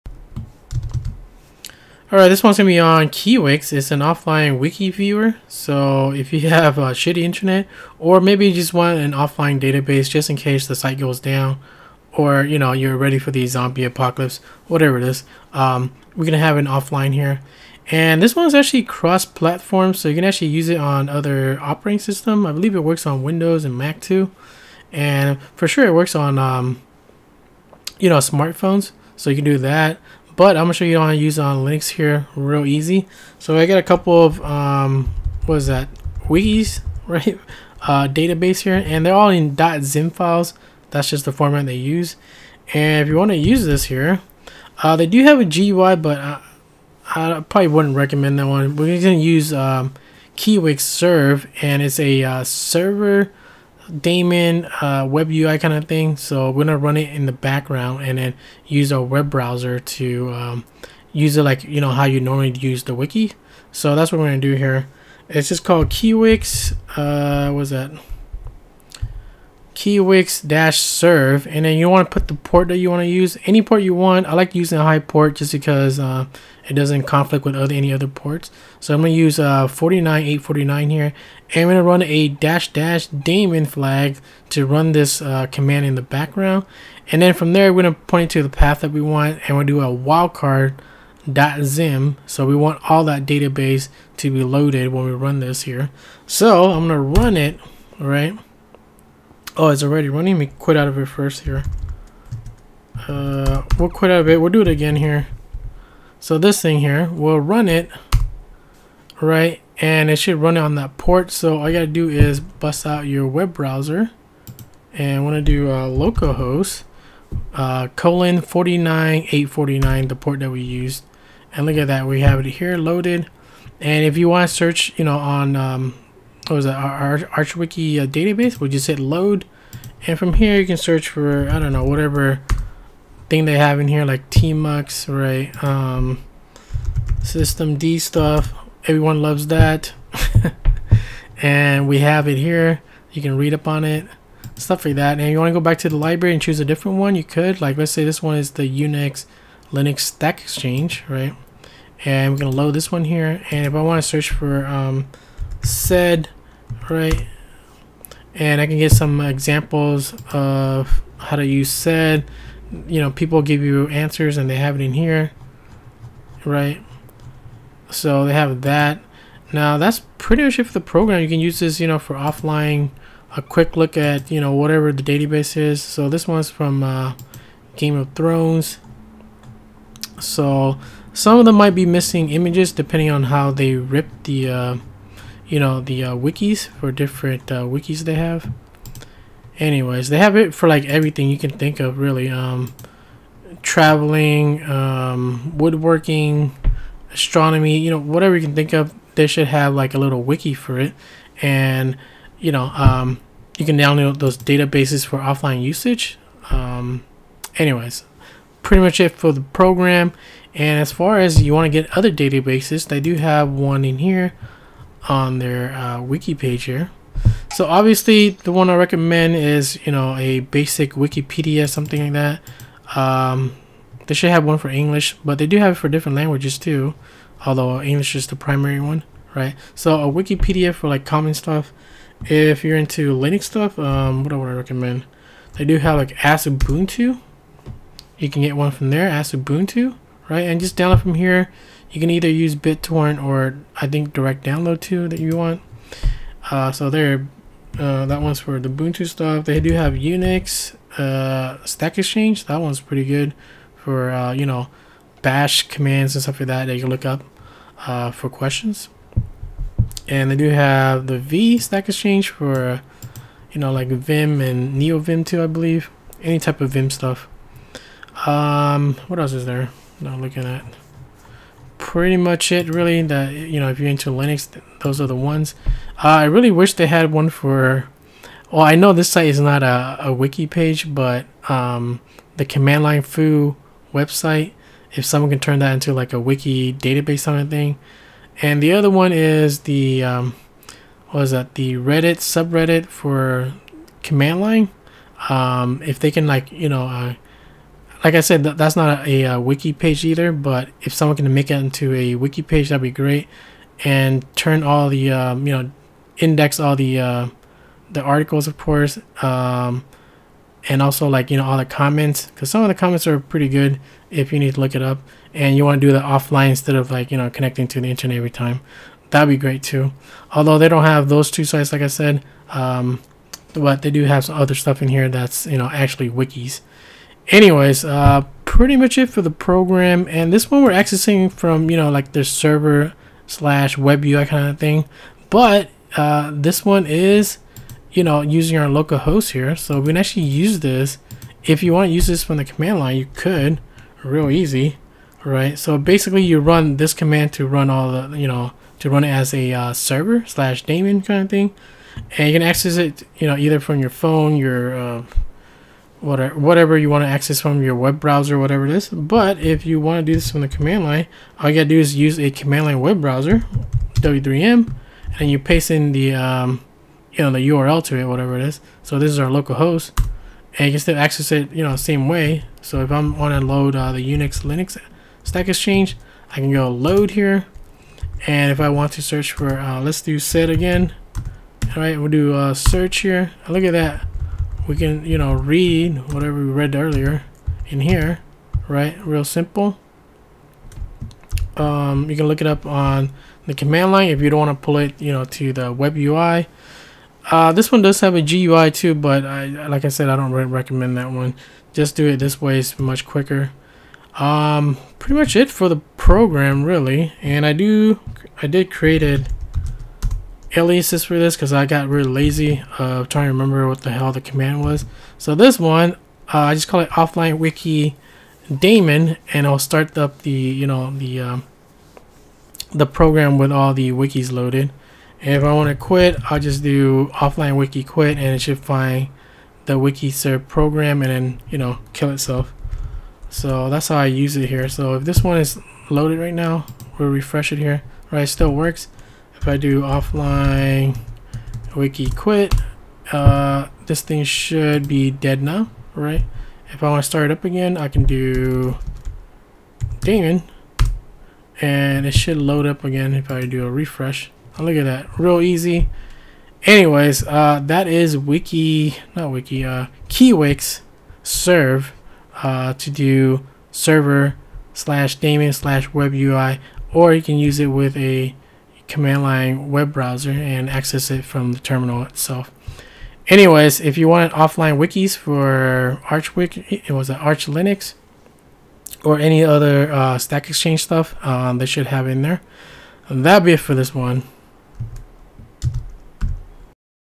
kiwix-serve -- Offline Wiki Viewer - Linux WEBUI
# commands used:

offlinewiki-daemon() { kiwix-serve --port 49849 --daemon /media/EdoTensei/wiki/*.zim ; }
offlinewiki-quit() { kill $(ps -ef | grep -i '[K]iwix-serve' | awk '{print $2}') ;}